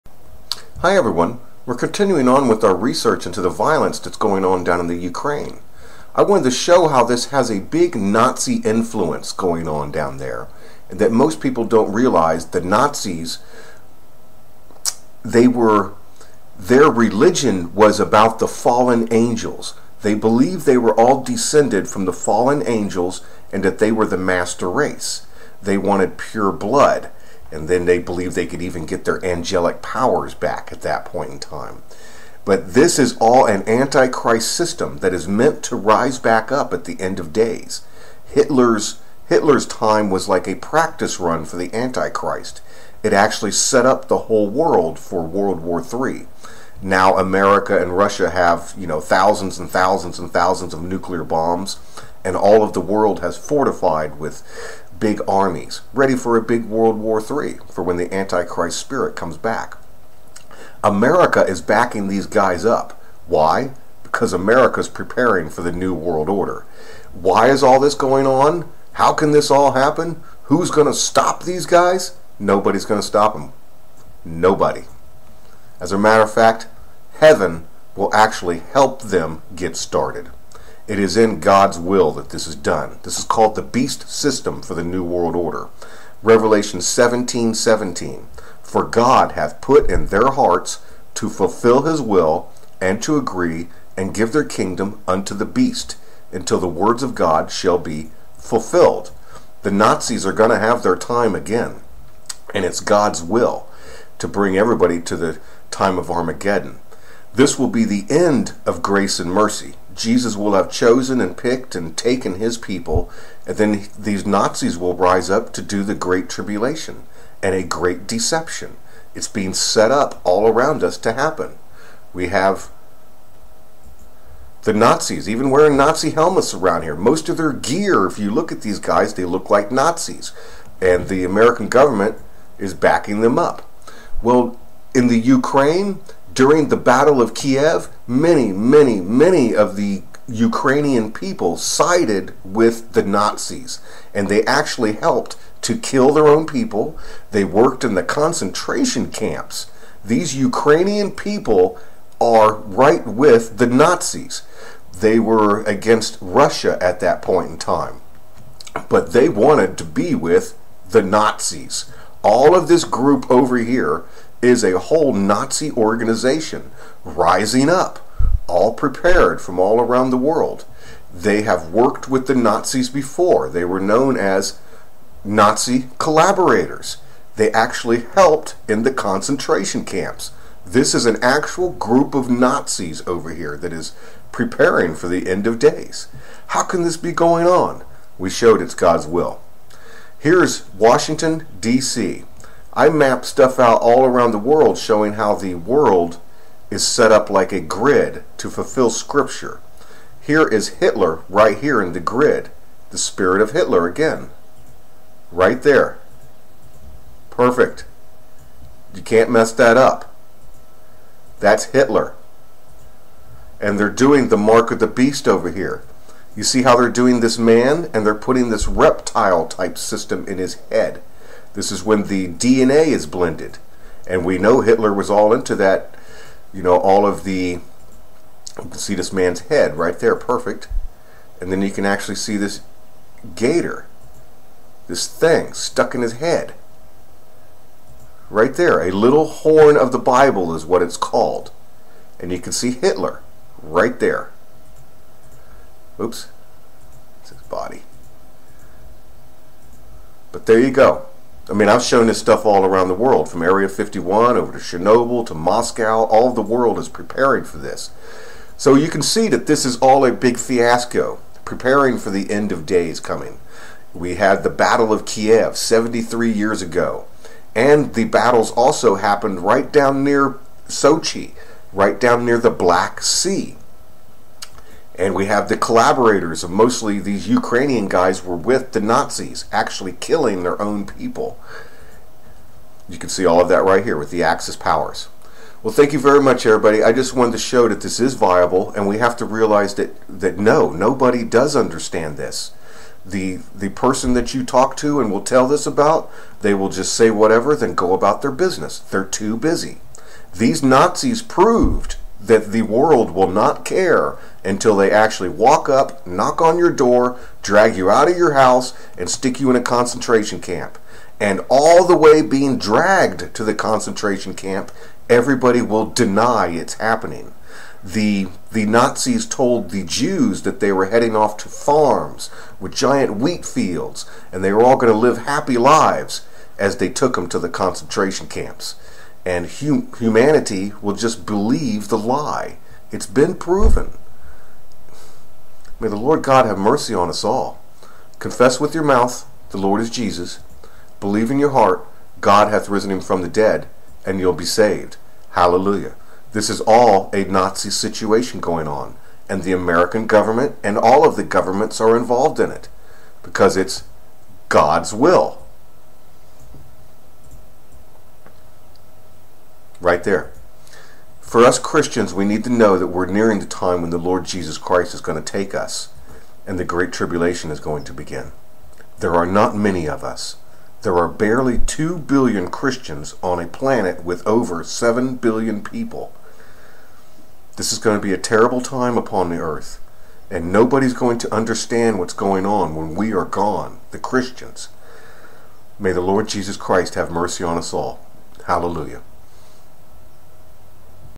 Ukraine Kiev Violence. The Nazis Rising up. Illuminati Freemason NWO Symbolism.
An investigative Look into the Protesters in Kiev and the Nazi Influence Running the Protest.
The NWO is ready to rise.

Under occupation[edit]

Main article: Reichskommissariat Ukraine
Some Ukrainians cooperated with the German occupiers, participating in the local administration, in German-supervised auxiliary police, Schutzmannschaft, in the German military, and serving as concentration camp guards. Nationalists in the west of Ukraine were among the most enthusiastic early on, hoping that their efforts would enable them to establish independent state later on. For example, on the eve of Barbarossa as many as four thousand Ukrainians, operating under Wehrmacht orders, sought to cause disruption behind Soviet lines. After the capture of Lviv, an important Ukrainian city, OUN leaders proclaimed a new Ukrainian State on June 30, 1941 and were simultaneously encouraging loyalty to the new regime, in hope that they would be supported by the Germans. Already in 1939, during the German-Polish war, the OUN had been "a faithful German auxiliary", according to[4]

Hallelujah.